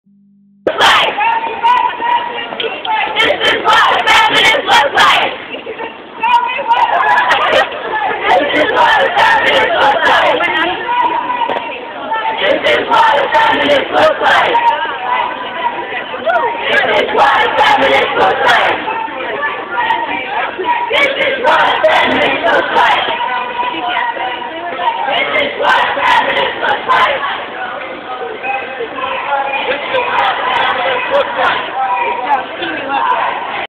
Tell me what a feminist looks like!
More footage from SlutWalk Providence, 9.17.11.